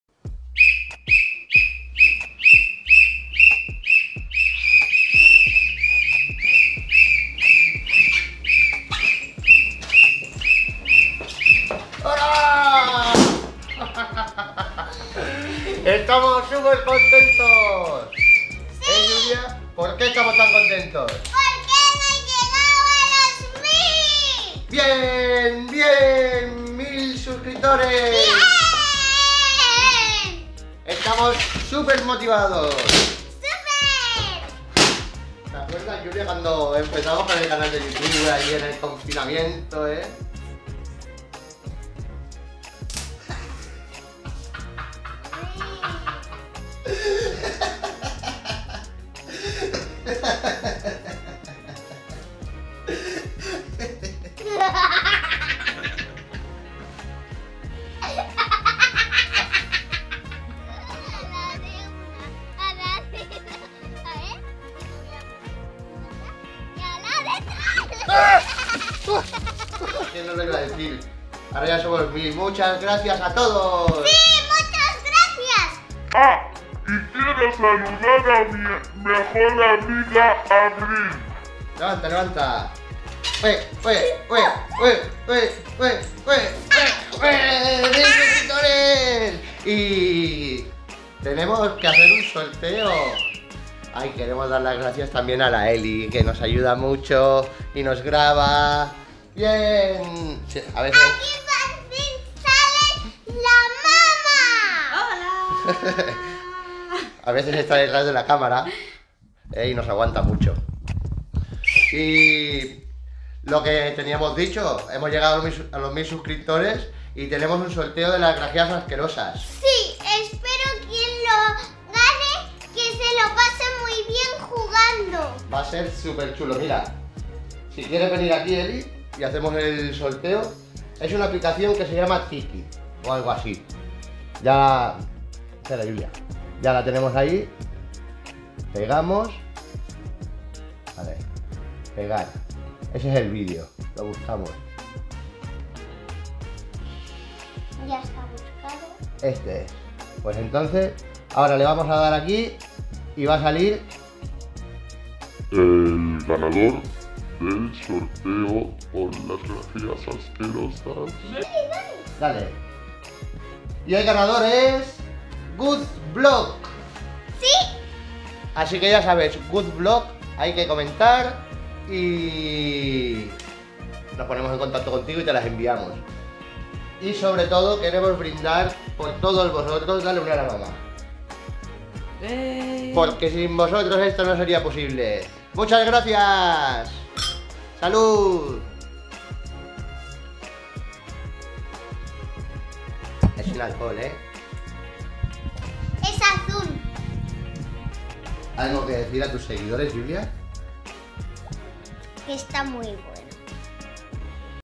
Especial 1000 SUSCRIPTORES‼️special 1000 SUSCRIBERS🥳🥳 sorteo de GRAGEAS ASQUEROSAS 🤢🤮
Video especial por nuestros mil suscriptores,sin vosotr@s esto no sería posible.
Como ya anunciamos en un video anterior hemos sorteado las grageas asquerosas que prometimos por llegar a los 1000 suscriptores.
GRACIAS‼️‼️